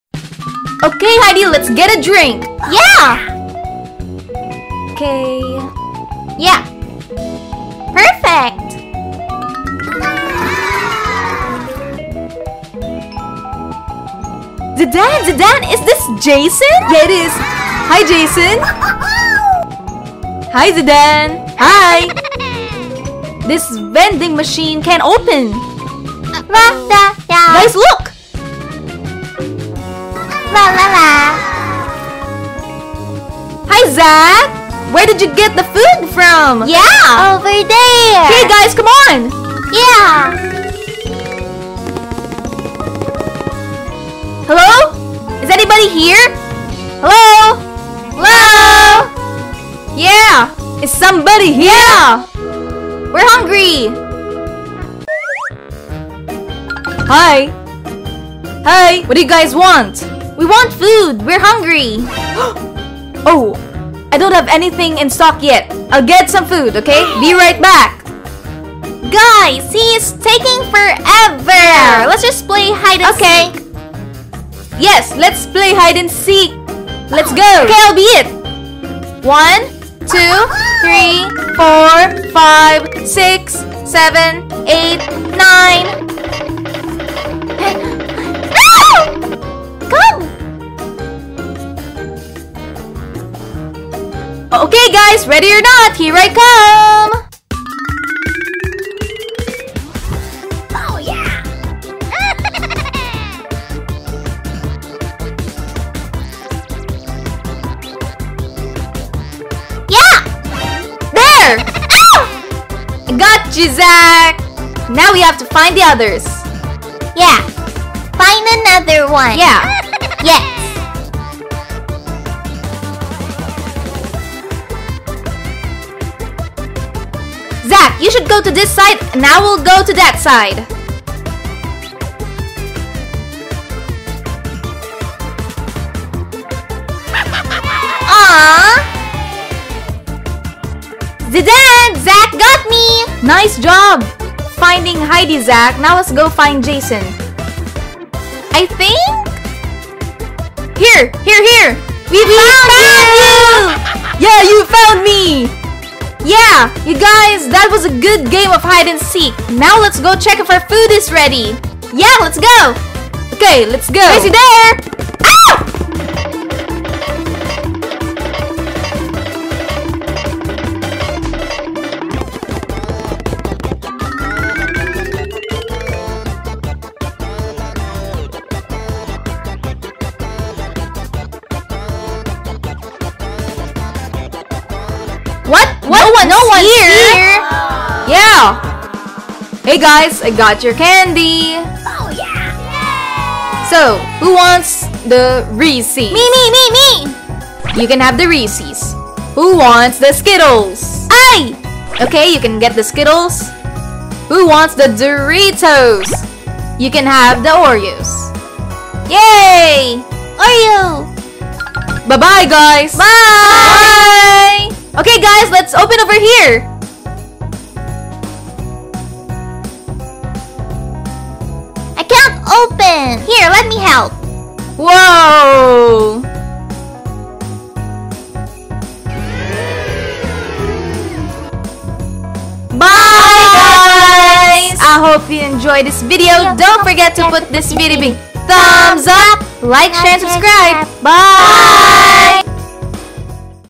Heidi dan Zidane aktivitas dalam ruangan untuk anak anak
Heidi dan Zidane mulai bosan di hotel. Jadi sebelum makan mereka memutuskan untuk memainkan permainan dalam ruangan yang menyenangkan - petak umpet.  Seri baru tentang permainan dan aktivitas dalam ruangan yang menyenangkan untuk dimainkan anak-anak di rumah dengan teman